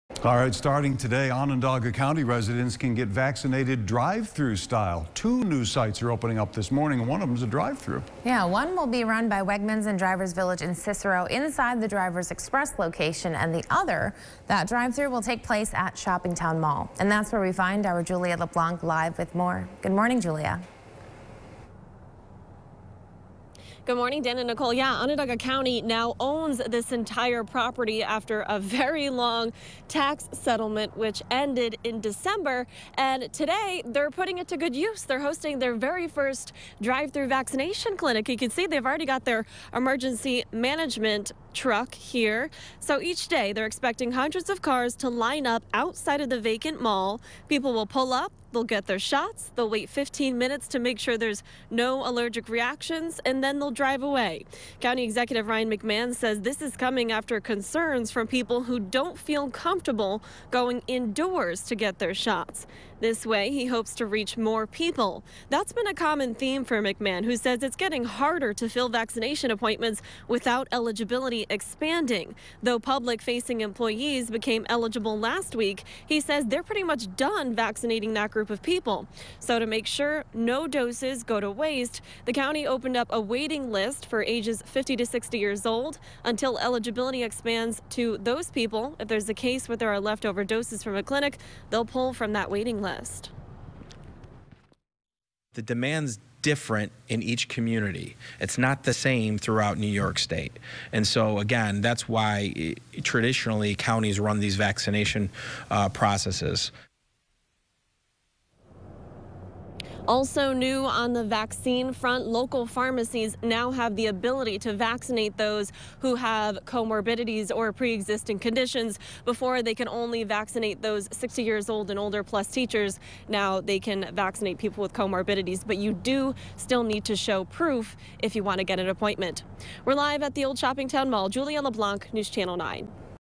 Drive-thru vaccine clinic at ShoppingTown 530 3/22/21
Onondaga County is hosting a drive-thru vaccination clinic today.